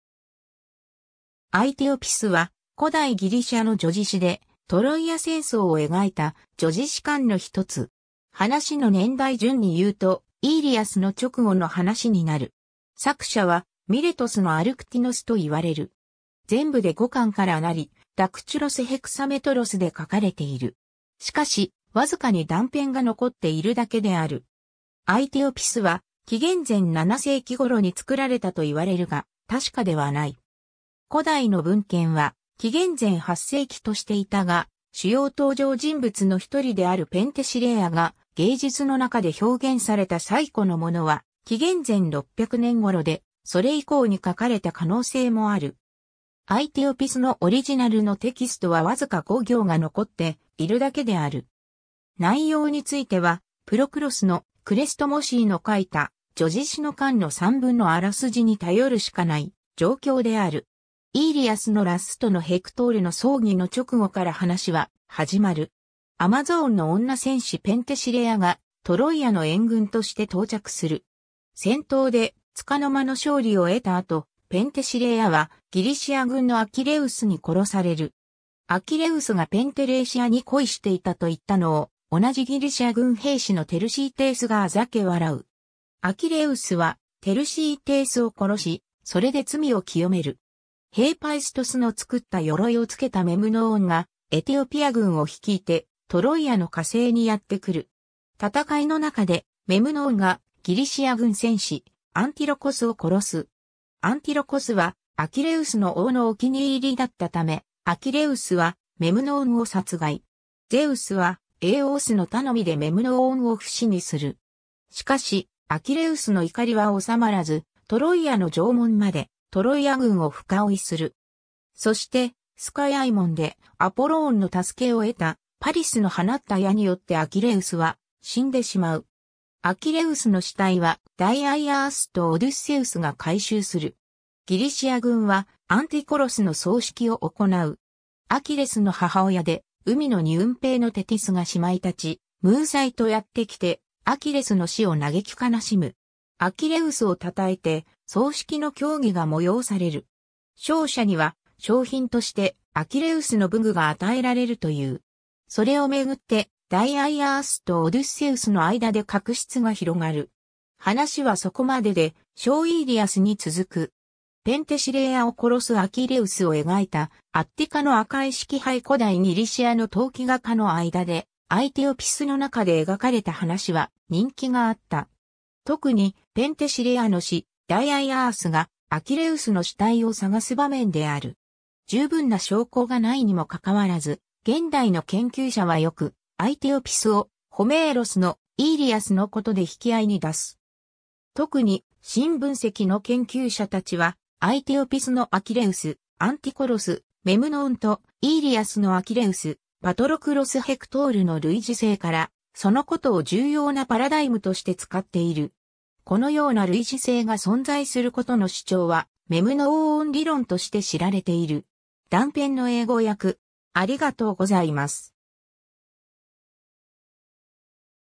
アイティオピス
『アイティオピス』（ギリシア語: Αἰθιοπίς, Aithiopis, ラテン語: Aethiopis）は、古代ギリシアの叙事詩で、トロイア戦争を描いた叙事詩環の1つ。
話の年代順にいうと『イーリアス』の直後の話になる。
作者はミレトスのアルクティノスと言われる。
全部で5巻から成り、ダクテュロス・ヘクサメトロス（長短短六歩格）で書かれている。
しかし、わずかに断片が残っているだけである。
 『アイティオピス』は紀元前7世紀頃に作られたと言われるが、確かではない。
古代の文献は紀元前8世紀としていたが、主要登場人物の1人であるペンテシレイアが芸術の中で表現された最古のものは紀元前600年頃で、それ以降に書かれた可能性もある。
 『アイティオピス』のオリジナルのテキストはわずか5行が残っているだけである。
内容については、プロクロスの『Chrestomathy』の書いた「叙事詩の環」の散文のあらすじに頼るしかない状況である。
 『イーリアス』のラストのヘクトールの葬儀の直後から話ははじまる。
 アマゾーンの女戦士ペンテシレイアがトロイアの援軍として到着する。
戦闘で束の間の勝利を得た後、ペンテシレイアはギリシア軍のアキレウスに殺される。
 アキレウスがペンテレイシアに恋していたと言ったのを、同じギリシア軍兵士のテルシーテースが嘲笑う。
アキレウスはテルシーテースを殺し、それで罪を浄める。
 ヘーパイストスの作った鎧をつけたメムノーン（エーオースとティートーノスの子）が、エティオピア軍（このエティオピアは、現在のエチオピアとする説と、現在のイスラエル、ヨルダン、エジプトの一部を含み、ヤッファを都としたフェニキアの王国とする説がある）を率いてトロイアの加勢にやって来る。
 戦いの中で、メムノーンがギリシア軍戦士アンティロコス（ネストールの子）を殺す。
アンティロコスはアキレウスの大のお気に入りだったため、アキレウスはメムノーンを殺害。
ゼウスはエーオースの頼みでメムノーンを不死にする。
しかしアキレウスの怒りはおさまらず、トロイアの城門までトロイア軍を深追いする。
そしてスカイアイ門で、アポローンの助けを得たパリスの放った矢によってアキレウスは死んでしまう。
アキレウスの死体は大アイアースとオデュッセウスが回収する。
 ギリシア軍はアンティコロスの葬式を行う。
 アキレスの母親で海のニュンペー（ニンフ）のテティスが姉妹たち、ムーサイ（ミューズ）とやって来て、アキレスの死を嘆き悲しむ。
 アキレウスを讃えて葬式の競技が催される。
勝者には賞品としてアキレウスの武具が与えられるという。
それをめぐって、大アイアースとオデュッセウスの間で確執が広がる。
 話はそこまでで、『小イーリアス』に続く。
 ペンテシレイアを殺すアキレウスを描いたアッティカの赤絵式杯（紀元前470年 - 紀元前460年頃） 古代ギリシアの陶器画家の間で『アイティオピス』の中で描かれた話は人気があった。
とくに、ペンテシレイアの死、大アイアースがアキレウスの死体を捜す場面である。
 十分な証拠がないにもかかわらず、現代の研究者はよく『アイティオピス』をホメーロスの『イーリアス』のことで引き合いに出す。
とくに「新分析」の研究者たちは、『アイティオピス』のアキレウス：アンティコロス：メムノーンと『イーリアス』のアキレウス：パトロクロス・ヘクトールの類似性から、そのことを重要なパラダイムとして使っている。
このような類似性が存在することの主張は「メムノーン理論（Memnon theory）」として知られている。
 断片の英語訳